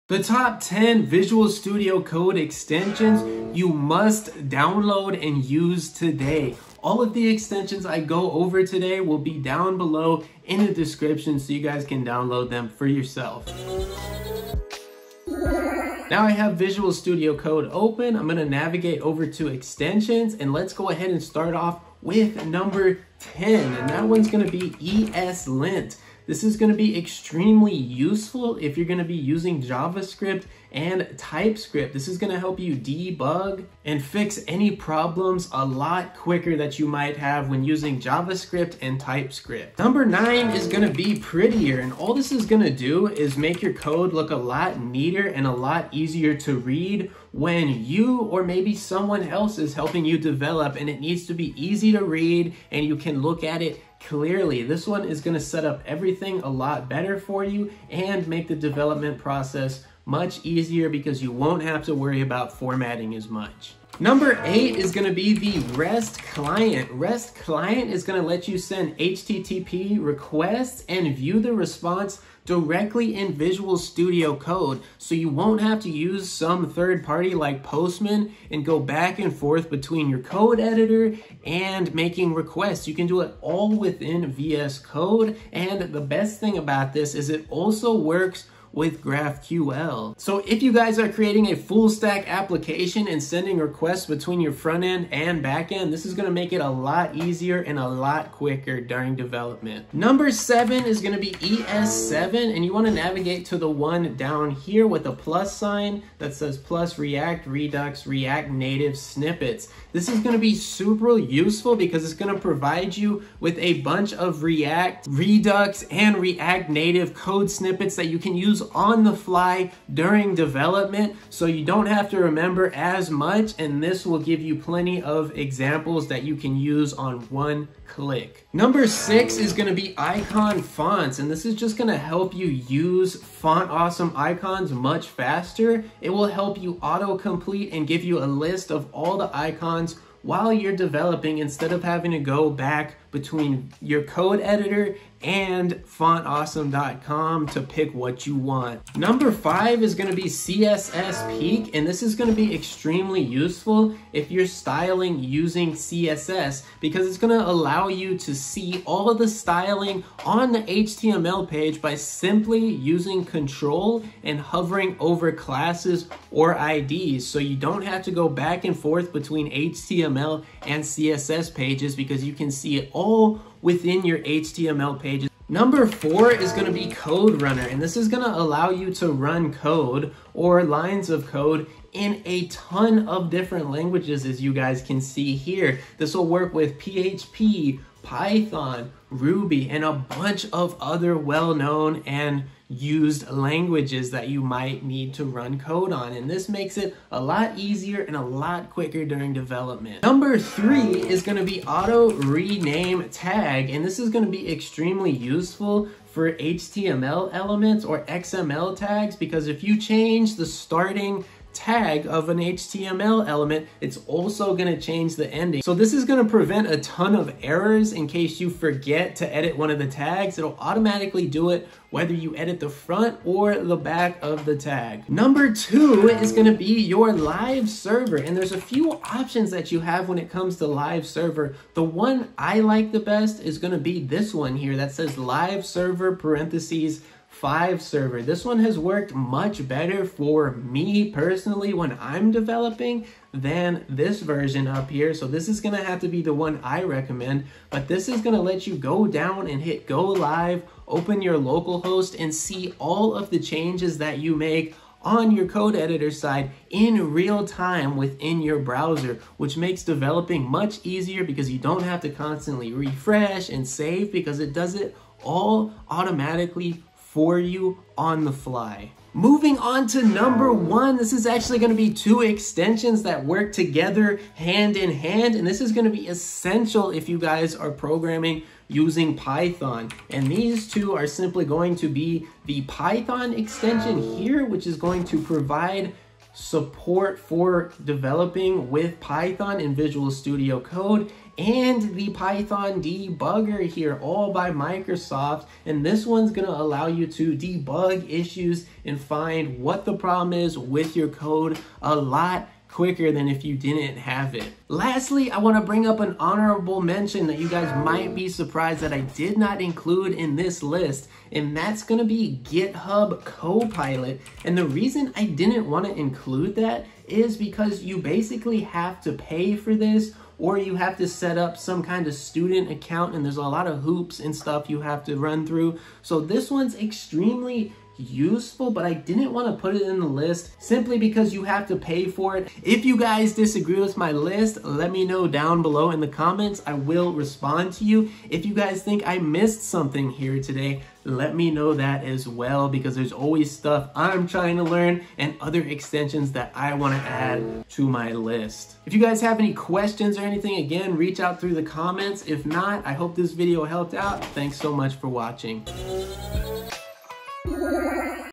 Top 10 VS Code Extensions for Developers | Visual Studio Code Tutorial
Top 10 Essential VS Code Extensions that every developer should have in their toolkit! Whether you're a beginner or an experienced programmer, these Visual Studio Code extensions will boost your productivity, streamline your workflow, and enhance your coding experience. From powerful debugging tools to time-saving shortcuts, we’ve handpicked the best extensions that will take your development to the next level. Perfect for web developers, frontend engineers, and anyone who loves writing clean, efficient code. Watch now to discover the must-have VS Code extensions and transform your coding setup!

Extensions:
10) ESLint | Finds JavaScript & TypeScript Problems
9) Prettier | Code Formatter
8) REST Client | REST Client allows you to send HTTP request and view the response in Visual Studio Code directly and works with GraphQL
7) ES7+ React/Redux/React-Native snippets | Provides a collection of shorthand snippets that can speed up your workflow when writing code for React, Redux, and React Native applications.
6) Icon Fonts | Easy access to font awesome icons in vs code
5) CSS Peak | See CSS Styling in HTML page w/ ctrl hover
4) Code Runner | Run code snippet or code file for multiple languages
3) Auto Rename Tag | Automatically rename paired HTML/XML tag,
2) Live Server (Five Server) | Local Dev Environment
1) Python & Python Debugger | Support for Python Dev and Debugging

Mentions
- GitHub copilot not mentioned because it is paid

0:00 - 0:21 - Intro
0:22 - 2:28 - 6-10
2:29 - 5:03 - 1-5
5:04 - 5:58 - Honorable Mentions & Outro